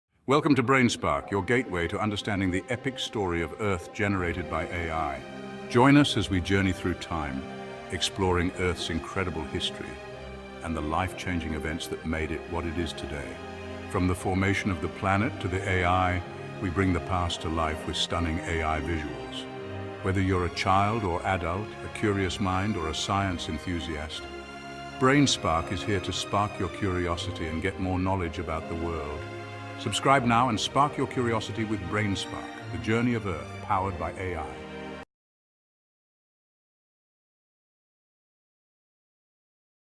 Welcome to Brain Spark! Uncover Earth's History with AI-Powered Learning 🌍✨
Discover Brain Spark, your new destination for exploring Earth’s incredible story through the power of AI! From the planet’s birth to epic climate changes and evolutionary milestones, our channel dives into the major events that shaped life on Earth. With captivating AI-generated visuals and engaging storytelling, each episode brings science, history, and curiosity to life.

Whether you’re a student, a lifelong learner, or just someone who loves science, Brain Spark makes learning fun and accessible for everyone. Ready to explore the wonders of our world in a whole new way?